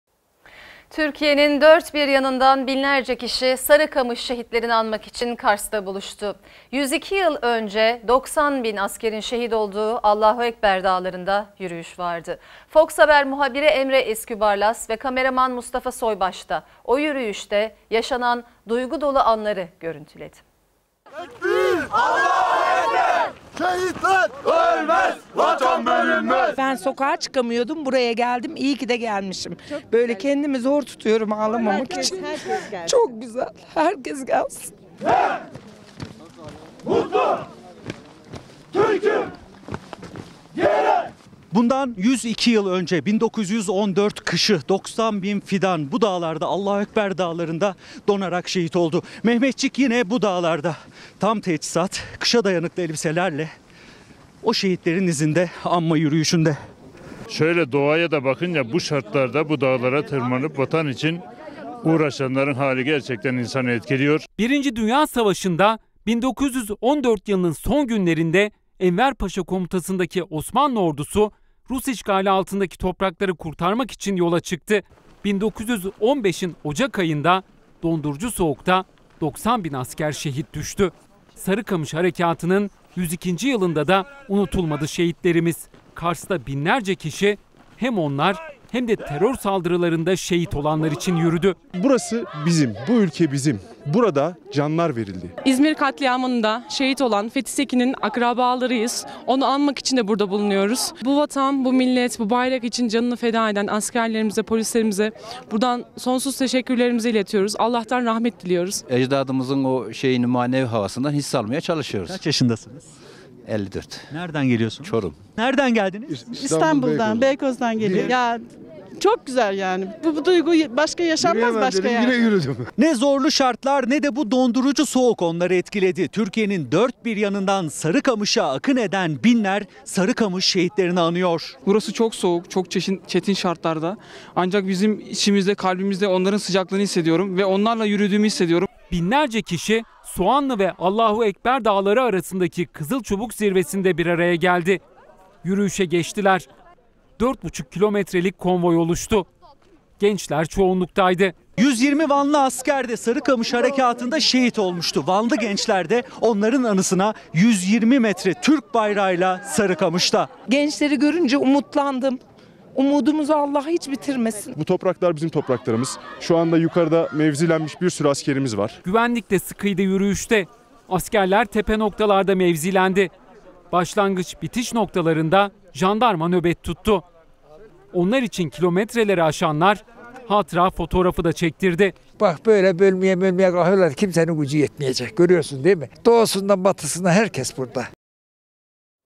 Binlerce kişi Sarıkamış şehitleri için yürüdü, şehit Fethi Sekin'in akrabaları da oradaydı
Kars Sarıkamış , 8 Ocak 2017
Muhabir : Emre İzkübarlas Kamera:Mustafa Soybaş